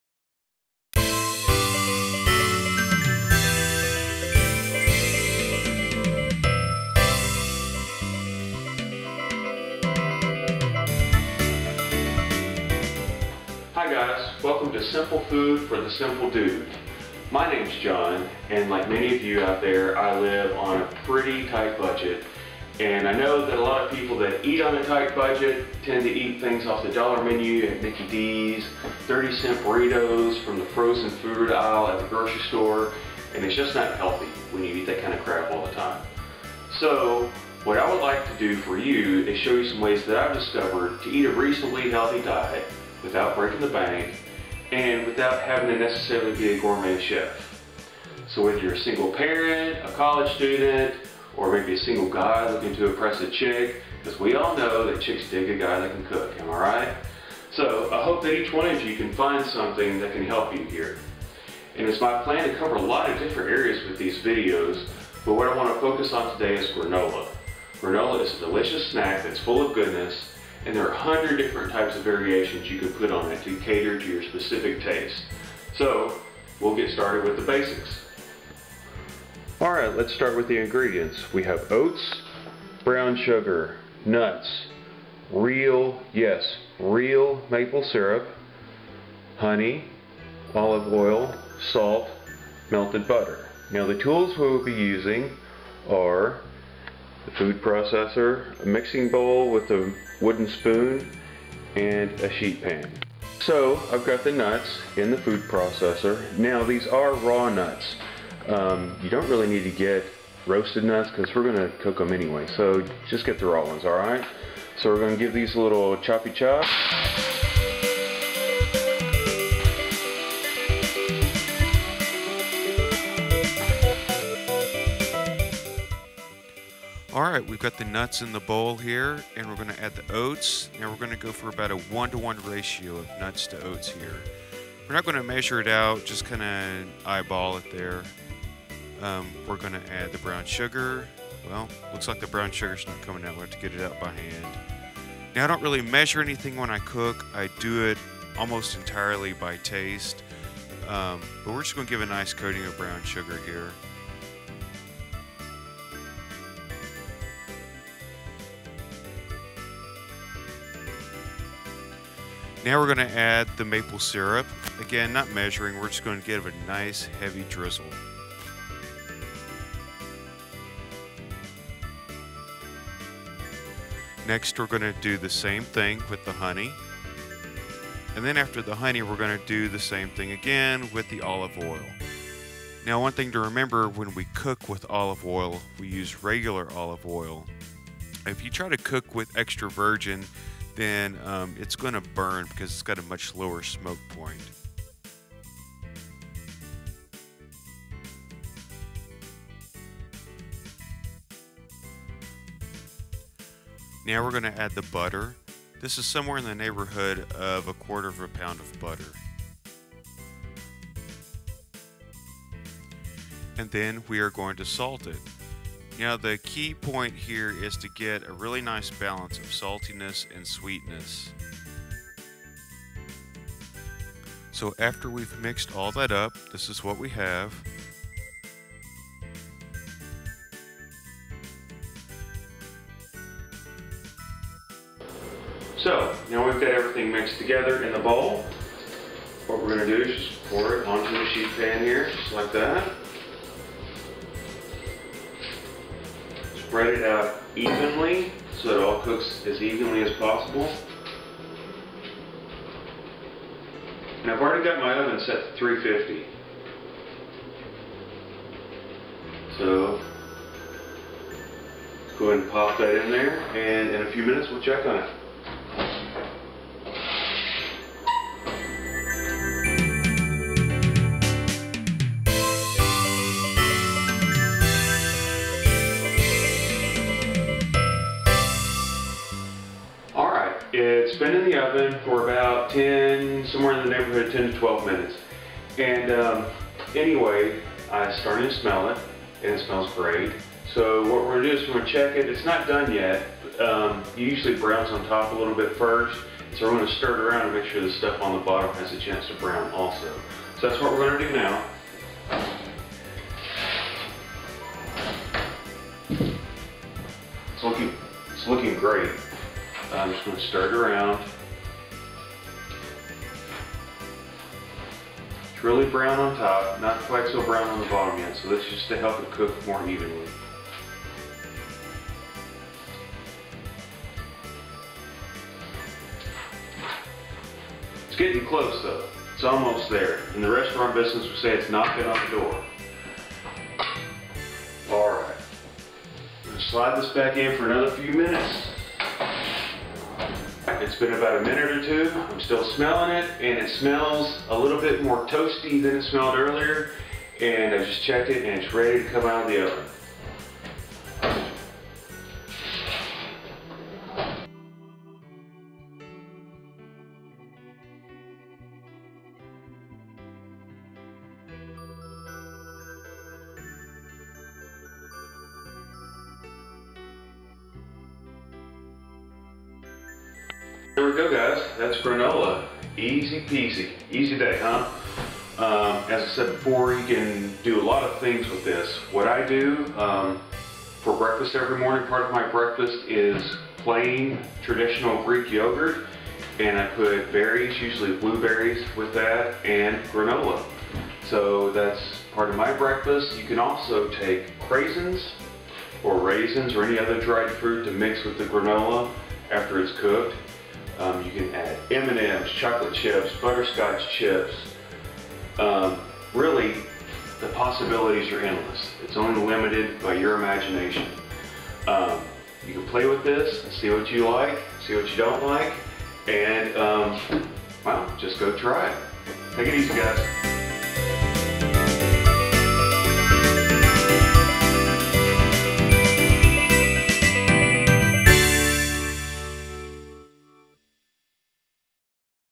Simple Food For The Simple Dude - Episode 1 Granola
Simple and inexpensive ways to eat well on a modest budget. Doing the best you can with what you have.